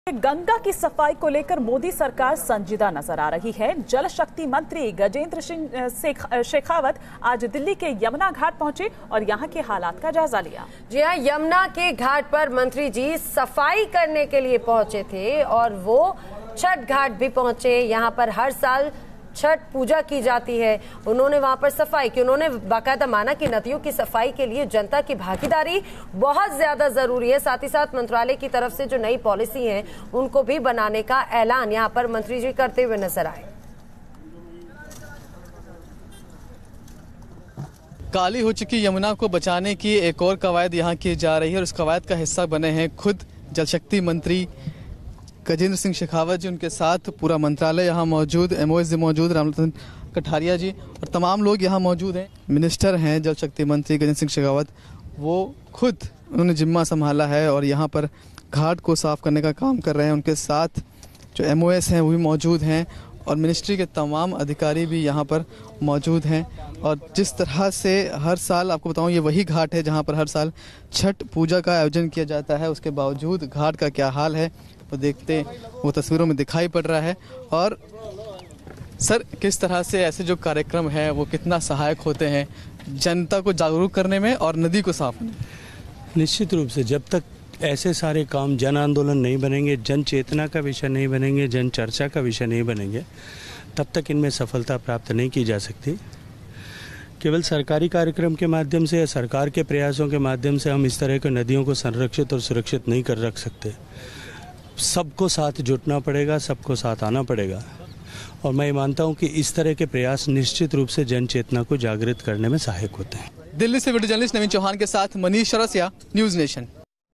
Jal Shakti Minister Gajendra Singh cleans Yamuna Ghat in Delhi
Newly appointed Jal Shakti Minister Gajendra Singh Shekhawat on Monday went to Yamuna Ghat in Delhi and participated in a cleaning drive. Shekhawat said that people's participation is crucial to keep our rivers clean and pollution free. Whether it was just a photo-op by the minister or he will seriously work to save the Yamuna would be seen in the coming days.